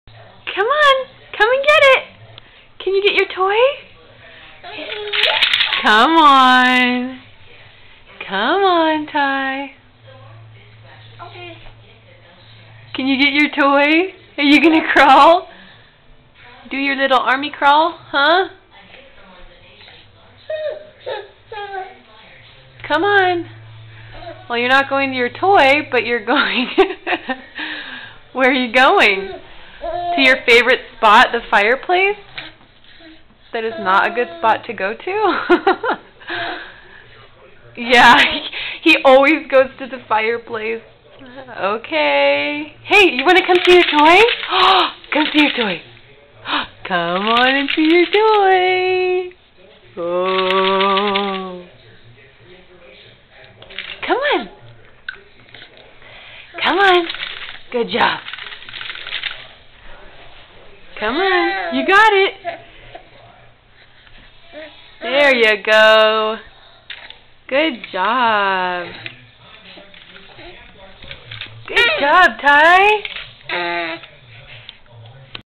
Army Crawl Ty
Ty is using his amazing army crawling skills to crawl all over the place.